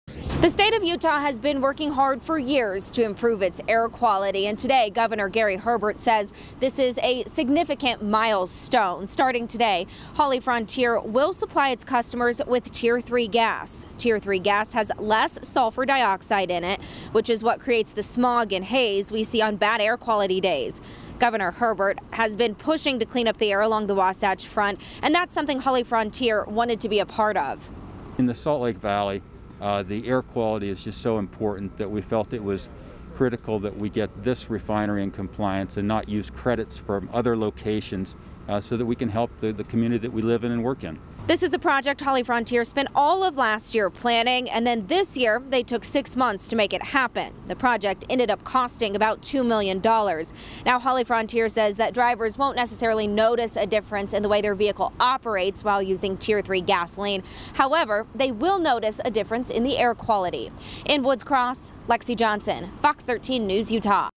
Holly Frontier Refinery begins delivering Tier 3 gas
Tier 3 gas is much better for air quality and the Holly Frontier refinery will now deliver it.